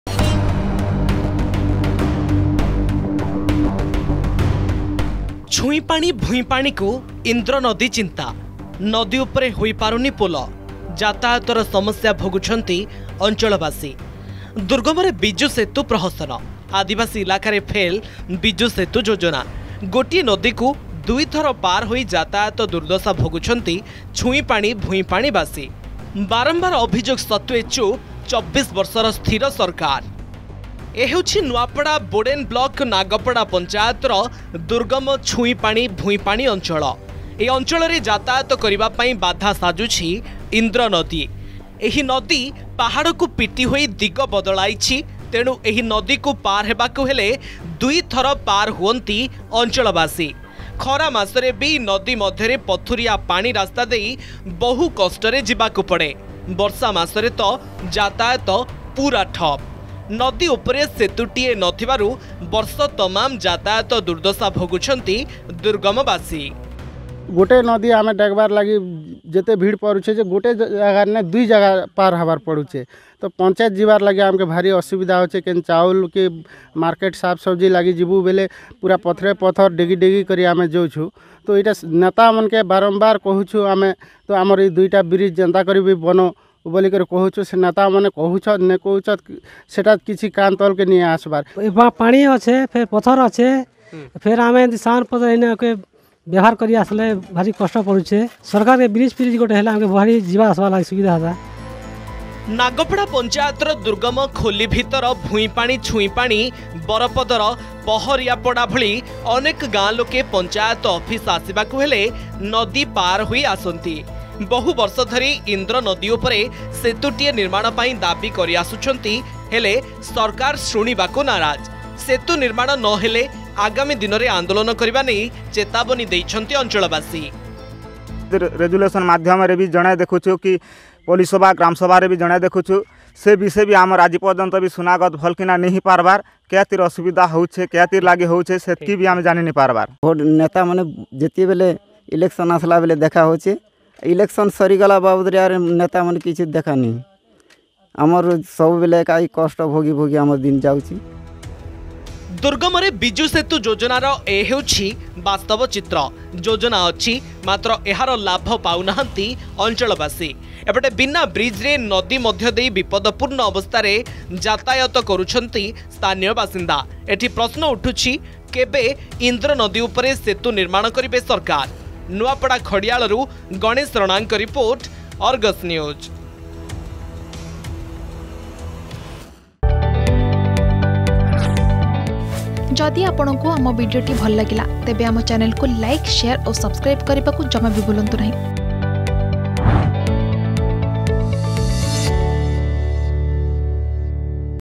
ଦୁର୍ଗମରେ ବିଜୁ ସେତୁ ପ୍ରହସନ
ଛୁଇଁପାଣି ଭୂଇଁପାଣିକୁ ଇନ୍ଦ୍ର ନଦୀ ଚିନ୍ତା। ନଦୀ ଉପରେ ହୋଇ ପାରୁନି ପୋଲ । ଯାତାୟତରେ ସମସ୍ୟା ଭୋଗୁଛନ୍ତି ଅଞ୍ଚଳବାସୀ। ଦୁର୍ଗମରେ ବିଜୁ ସେତୁ ପ୍ରହସନ ।